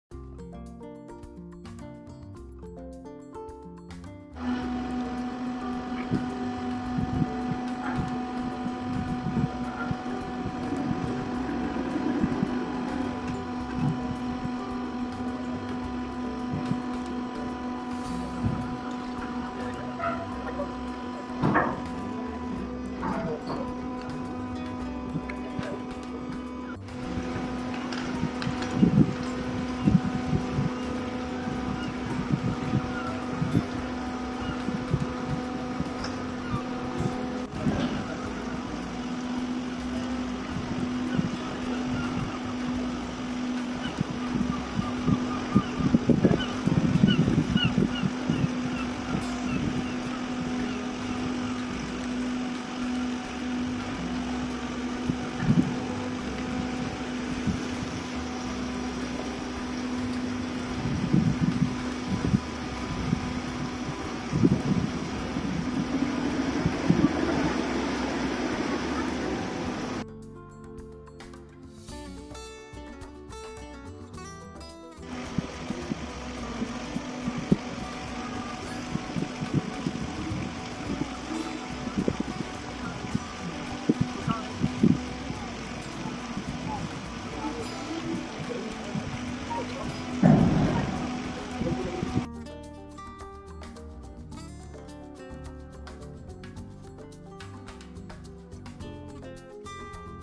A yacht leaving Whitehaven harbour
Watch a yacht leave Whitehaven harbour and start it's journey on the Irish Sea.